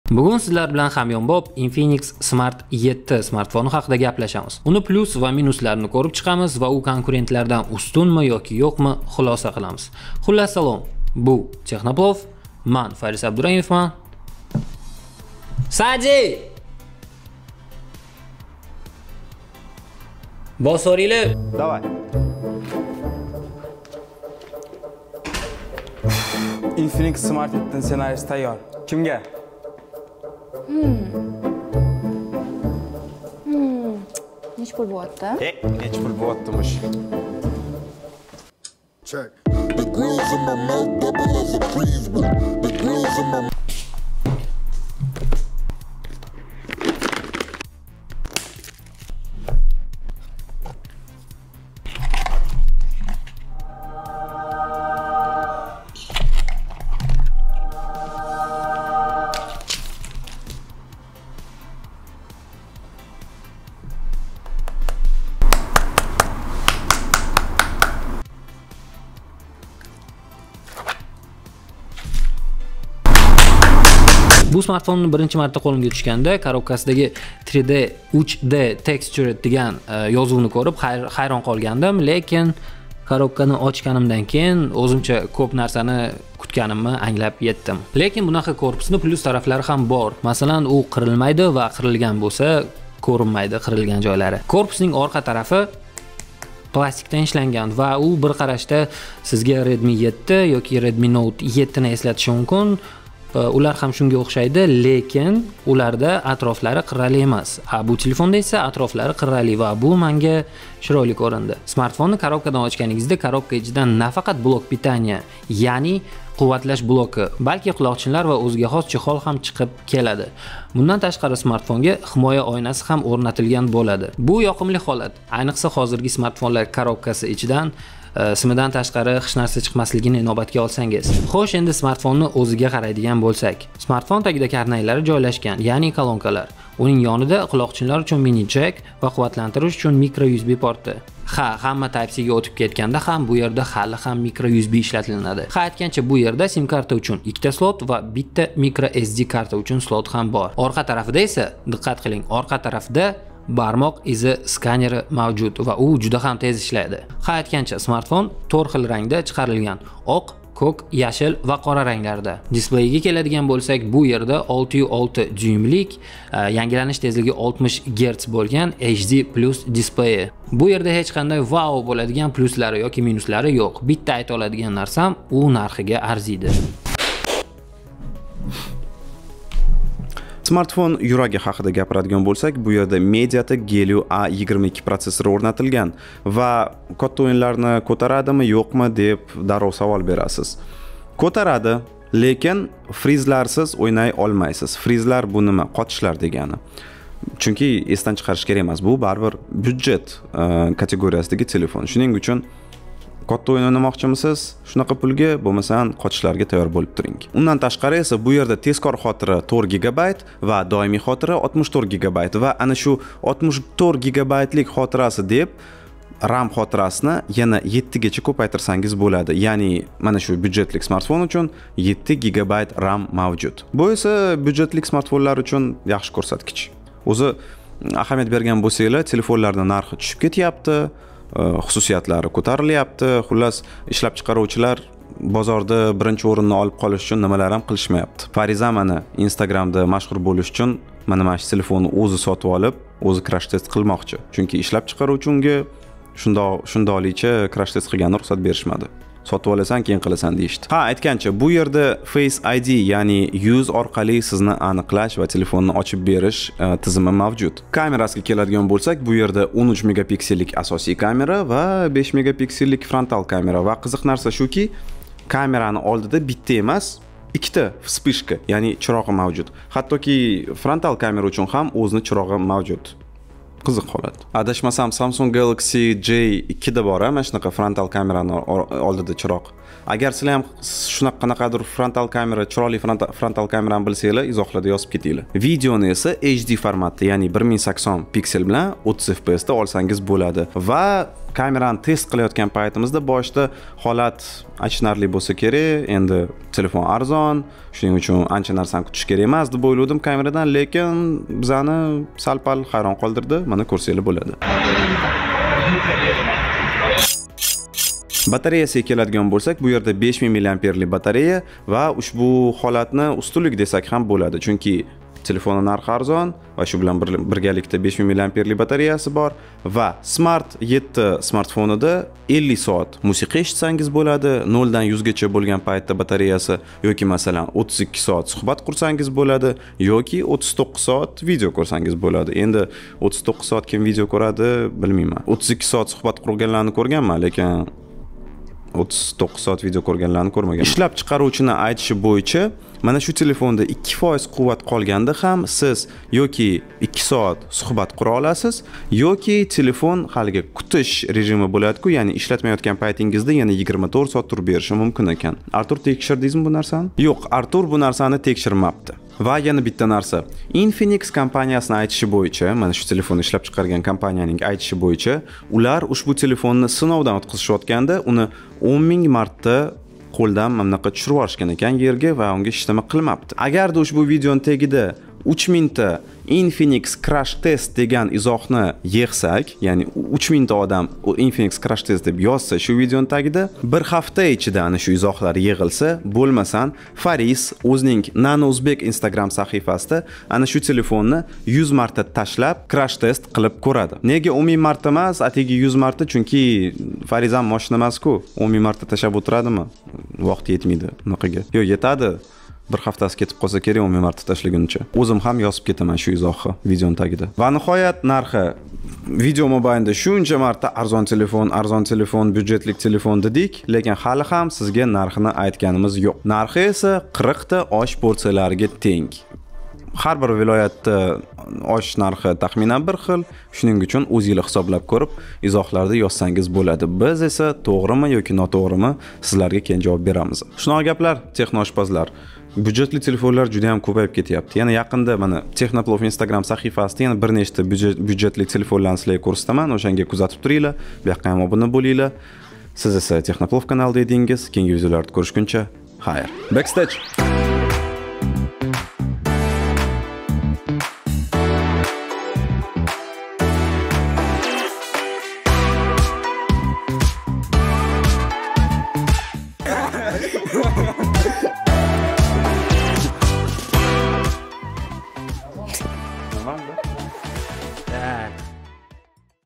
Eng arzon smartfon Infinix Smart 7 | TEXNOPLOV
00:00 - Kirish
00:20 - Sadi bos
00:39 - Unboxing
01:14 - Dizayn
03:01 - Smartfon ko'rsatkichlari
04:50 - Kamera
05:05 - Batareya
06:00 - Fariz crash test qiladimi?
06:55 - Narxi
07:26 - Backstage